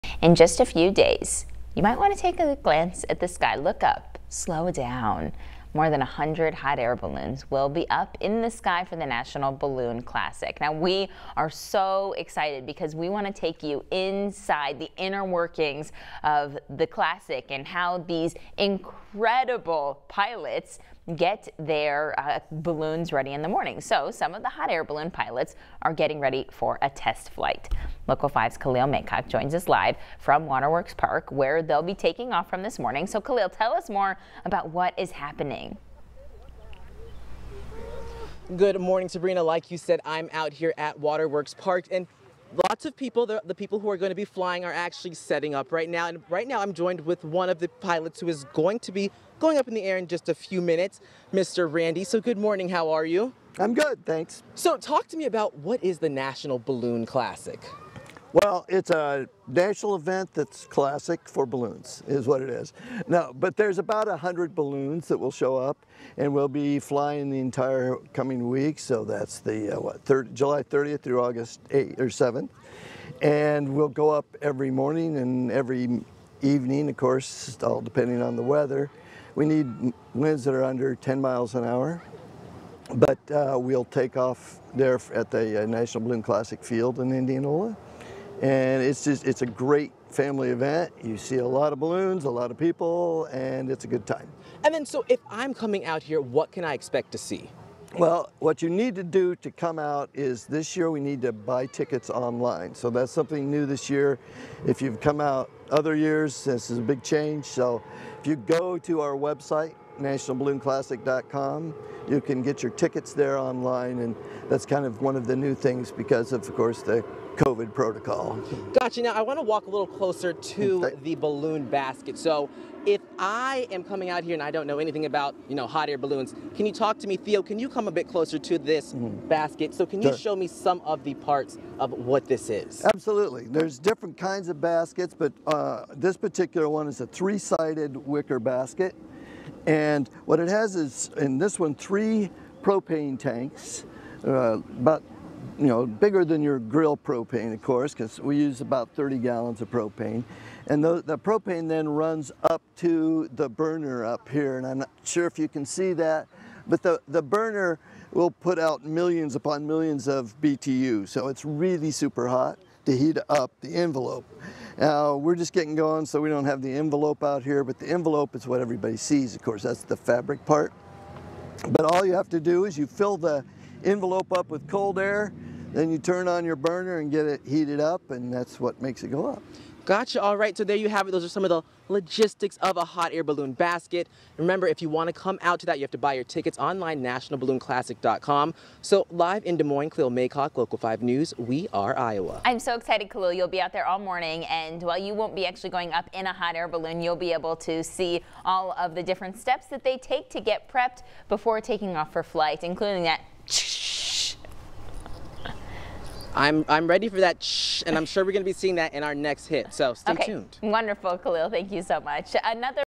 Hot air balloon pilot discusses upcoming National Balloon Classic
Randy Stone, a hot air balloon pilot, explains what the  National Balloon Classic is.